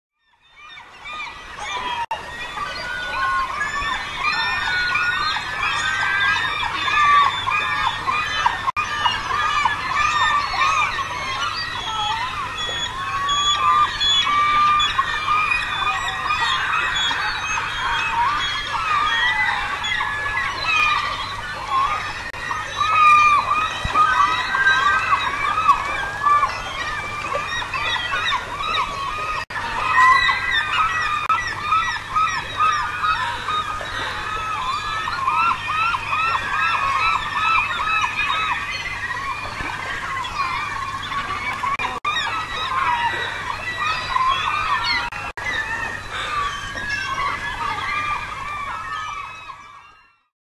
Fishing Wolf Pack
We're raising funds right now to support more videos like these.

Young wolves fish for pink salmon in the Great Bear Rainforest. October 2013.